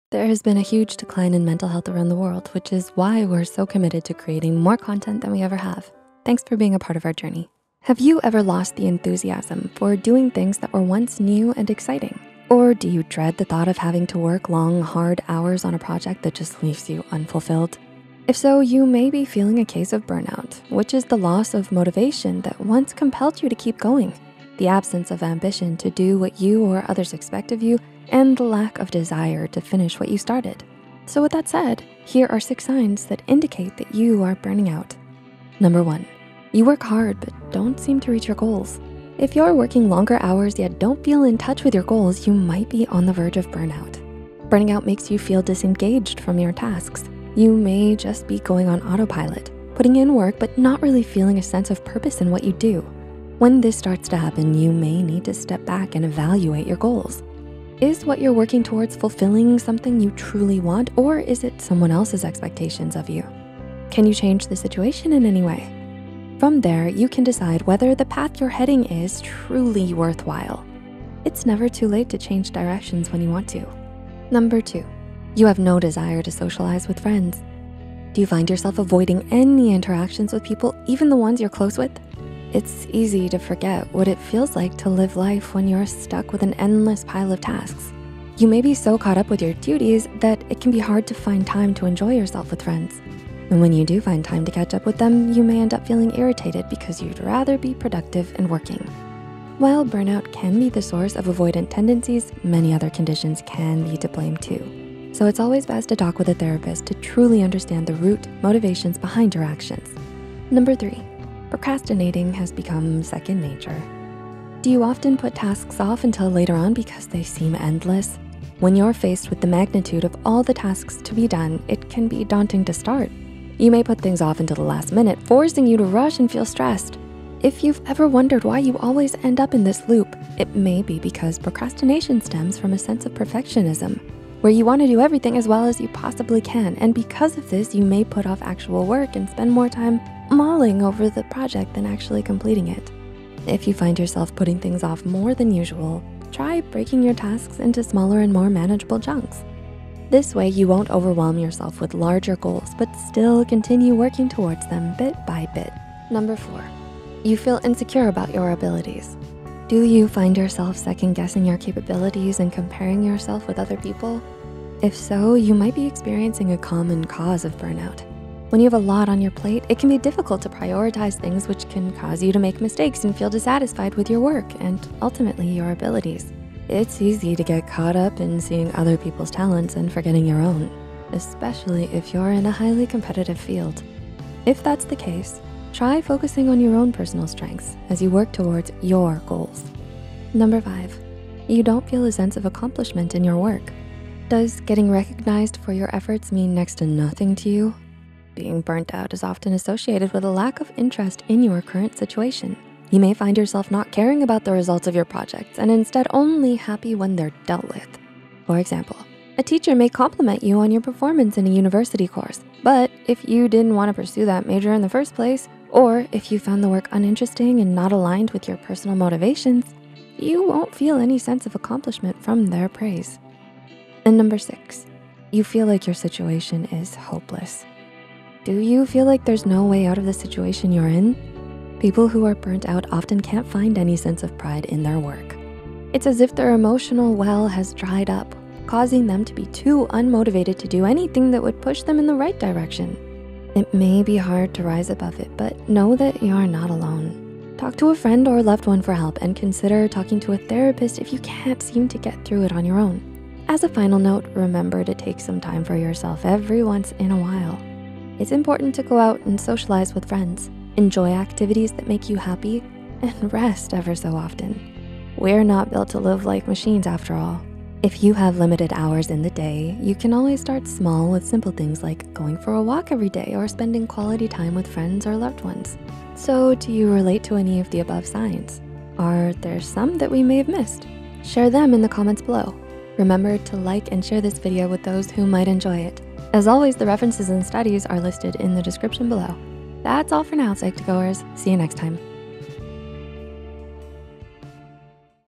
6 Subtle Signs You're Burning Out Without Realizing It
Has your mental health taken a hit lately? You could be very likely stressed and emotionally burnout. Burn out is a serious emotional and physical condition where you simply do not have the energy to do things anymore. Everything becomes a chore and you lose motivation for whatever you are doing. It's very common especially now, with covid and always having to stay home, and economic stress, but hopefully, you will recognize some of these burn out signs in someone who you care about.

Writer: Joshua Munoz
Script Editor: Caitlin McColl
Script Manager: Kelly Soong
Animator: Lesly Drue
YouTube Manager: Cindy Cheong
Thumbnail by: Billie

References
Smith, M. and Robinson, L. (Nov, 2021) Burnout Prevention and Treatment. Help Guide.
Ratini, M. (Aug 4, 2021) Signs You’re Burned Out.
Scott, E. (March 2020) Burnout Symptoms and Treatment.